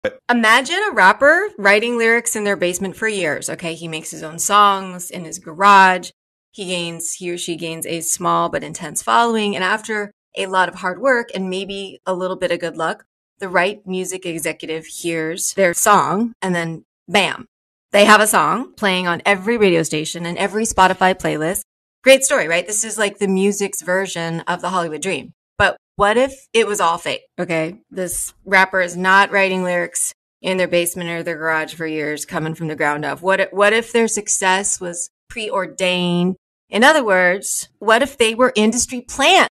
S2e10 Industry Plant with Jennifer Field
What’s an industry plat? Host Jennifer Field breaks it down for us.

Hear more about who is and who isn’t an industry plant on our latest episode starring screen and stage writer A. Zell Williams, Jennifer Field, Dwayne Perkins, and Koji Steven Sakai.